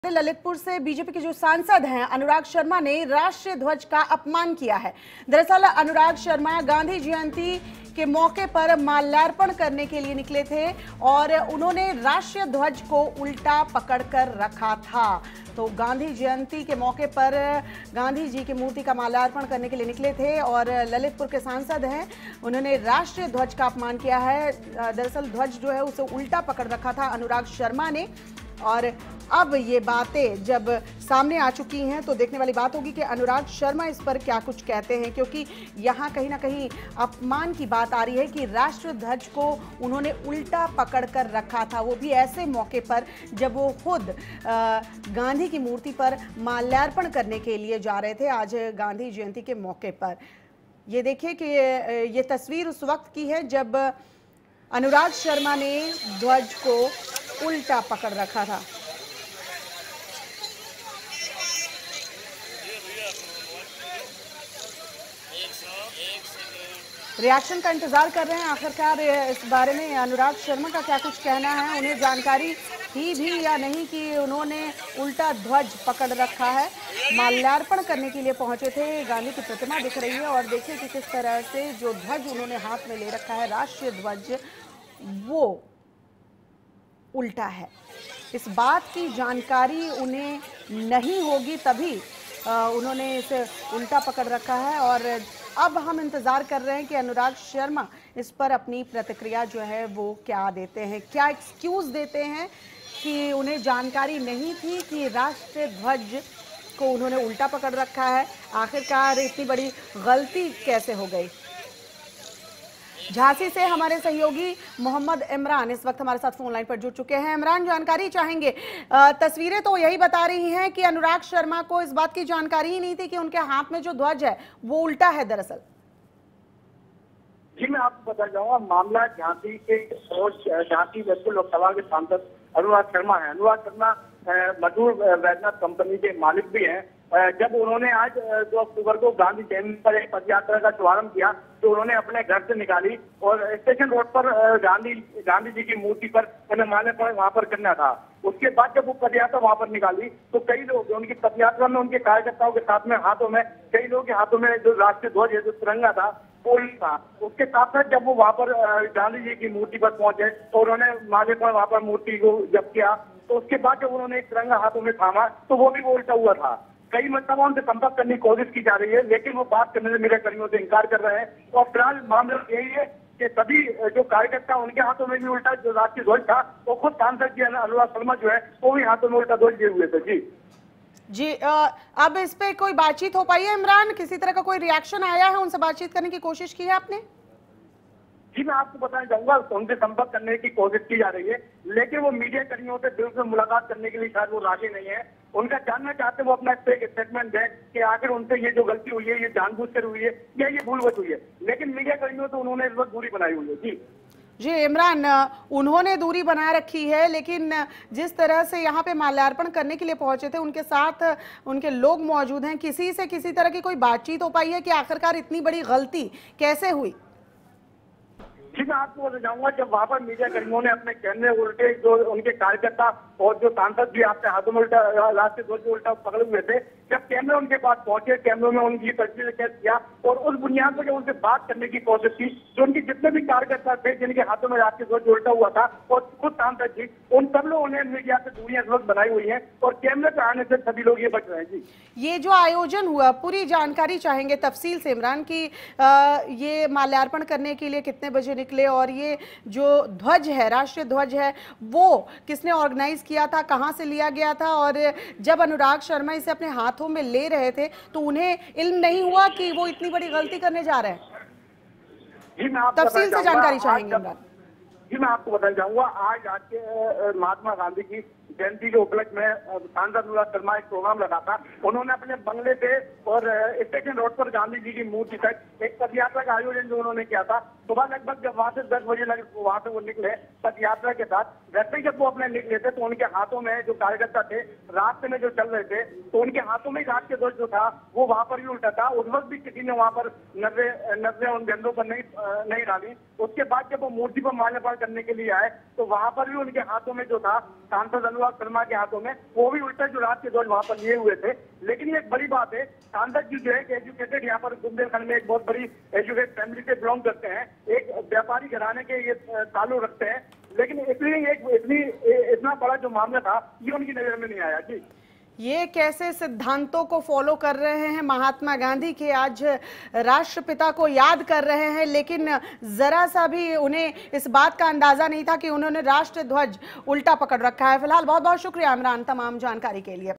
BJP सांसद अनुराग शर्मा ने किया राष्ट्रीय ध्वज का अपमान!!
UP में ललितपुर से बीजेपी सांसद अनुराग शर्मा ने राष्ट्रीय ध्वज का अपमान किया... गांधी जयंती पर माल्यार्पण करने के लिए अनुराग निकले थे...